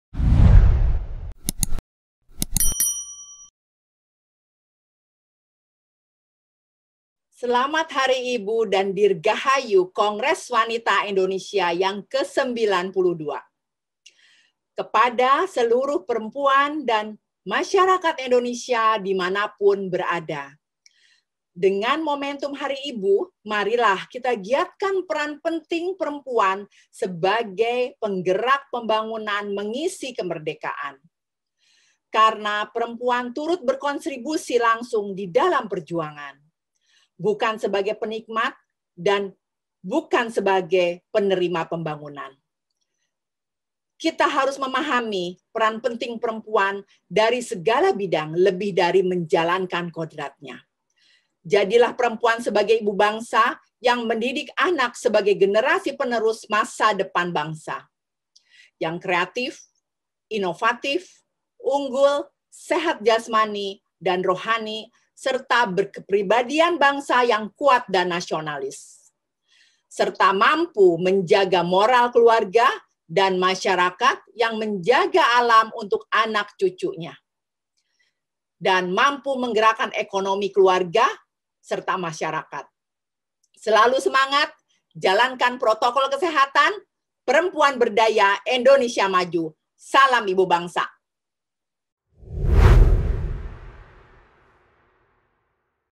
UCAPAN HARI IBU OLEH KETUA KONGRES WANITA INDONESIA
Ucapan selamat hari ibu dan dirgahayu kongres wanita Indonesia yang disampaikan oleh ketua Kongres Wanita Indonesia Dr. Ir. Hj. Sri Woerjaningsih, M.Pd.